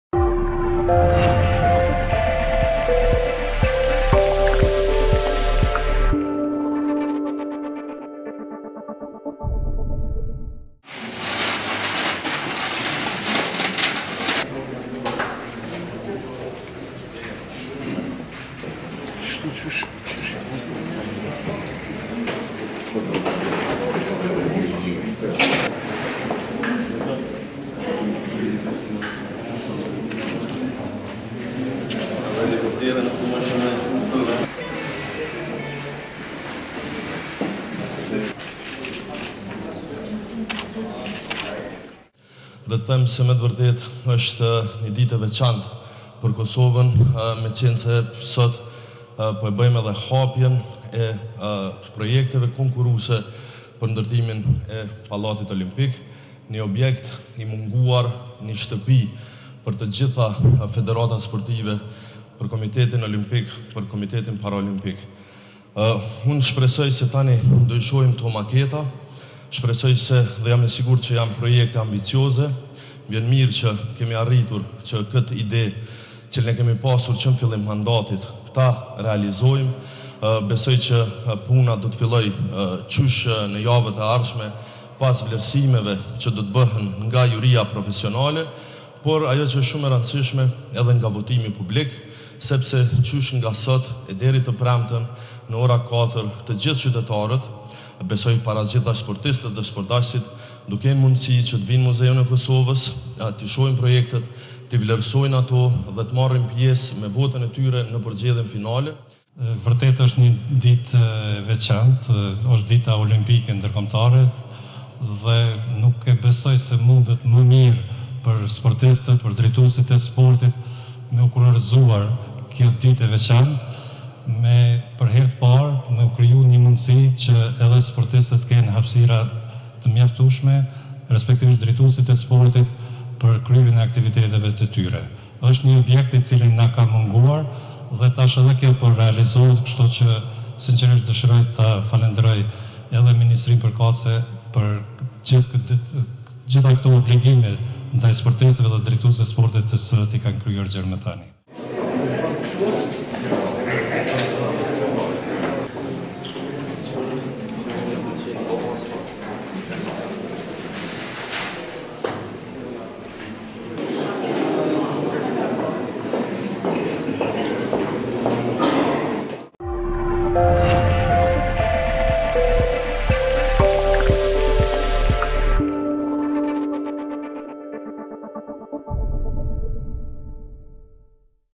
Kosova me Pallat Olimpik, brenda dy vitesh
Kosova do të bëhet me Pallat Olimpik më së largu deri më 23 qershor të vitit 2016. Ministria e Kulturës, Rinisë dhe Sportit ka hapur sot konkursin e ndërtimeve konkurruese për ndërtimin e pallatit olimpik. Ministri Memli Krasniqi e quajti këtë projekt si 'shtëpi e munguar' deri më tani.